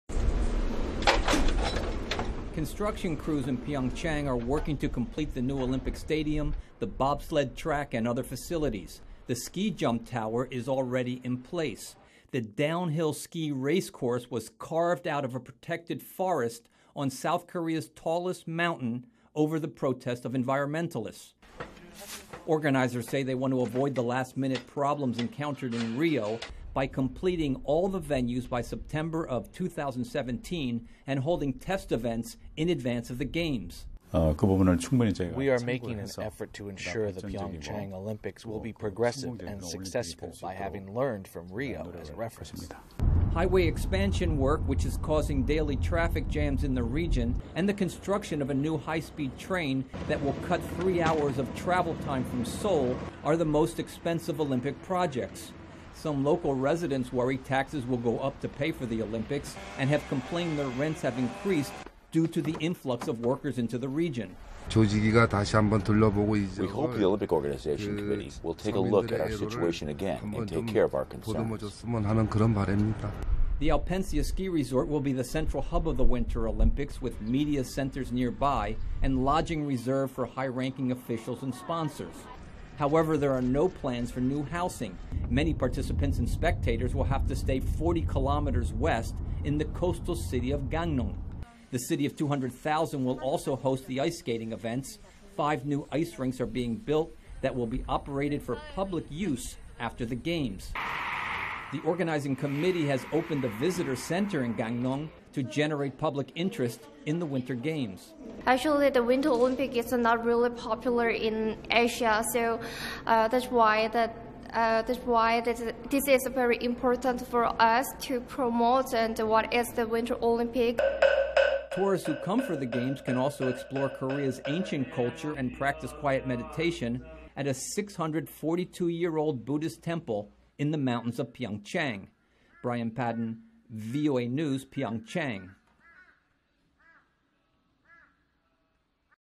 Pyeongchang Olympics Working to Avoid Rio Mishaps
Organizers of the 2018 Winter Olympics in South Korea are working to avoid the criticisms of delays and disorganization that have plagued the Rio games.